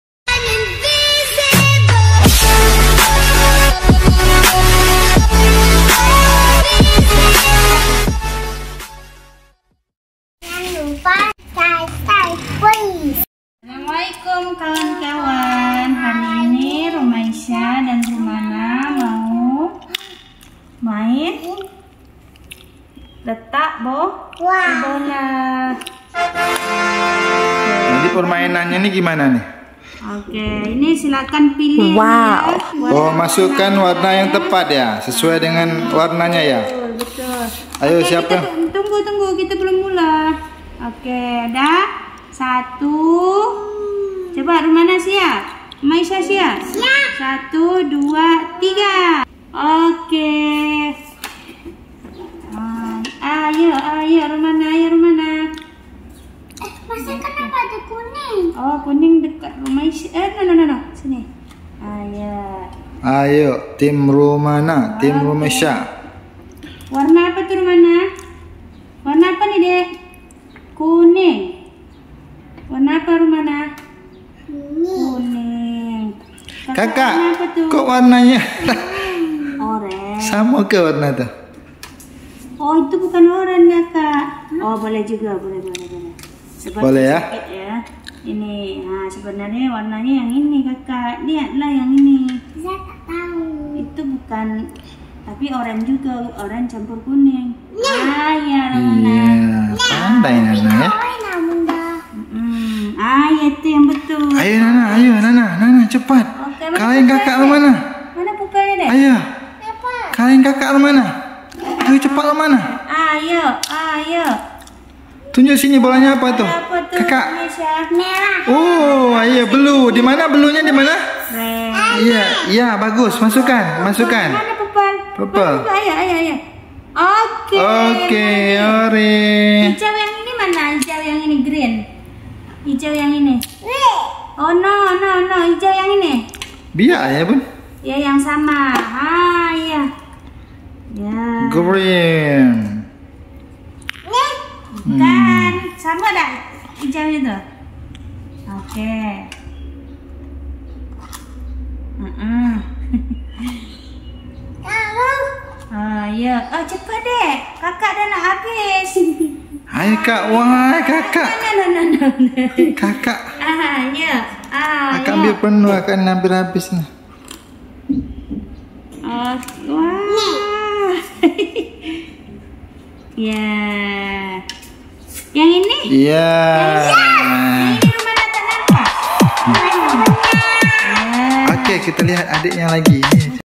permainan edukasi untuk anak mengenal warna
permainan cocokin warna untuk anak bikin mereka gembira

cara memperkenalkan warna kepada anak, permainan ini sangat baik untuk menambah kecerdasan anak

komen videonya ya, nanti rumana akan bikin vidio yang lebih seru lagi